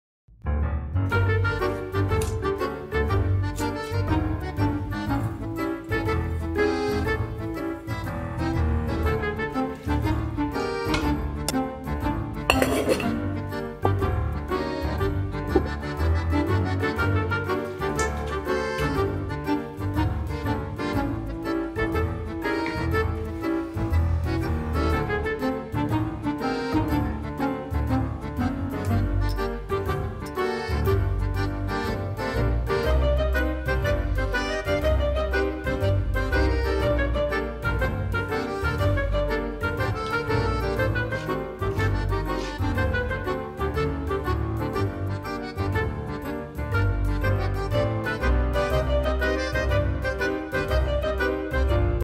Easy Cheap Broccoli Cheese Soup
This is the a super quickest and easy way to make broccoli soup from scratch. You almost need nothing but broccoli, cheese, oily, salt and walnuts.